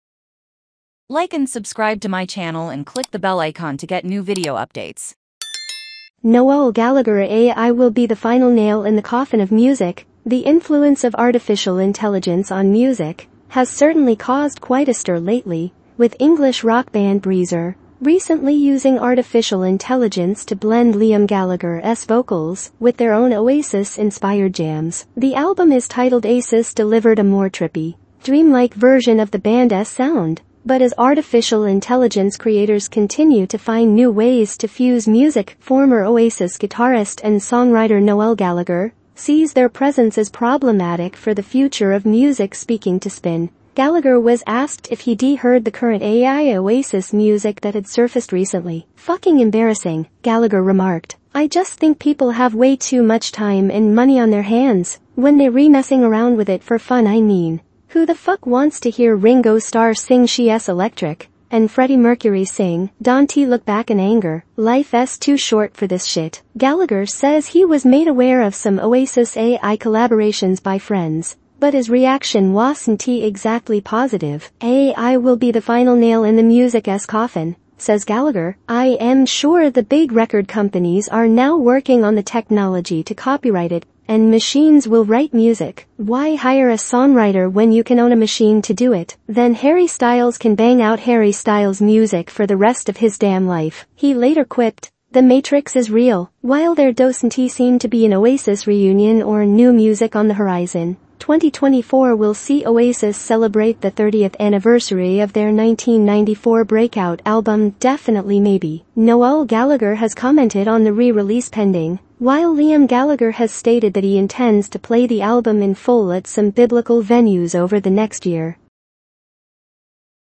NOEL GALLAGHER – AI WILL BE THE FINAL NAIL IN THE COFFIN OF MUSIC
The influence of artificial intelligence on music has certainly caused quite a stir lately. with English rock band Breezer recently using artificial intelligence to blend Liam Gallaghers vocals with their own Oasis inspired jams.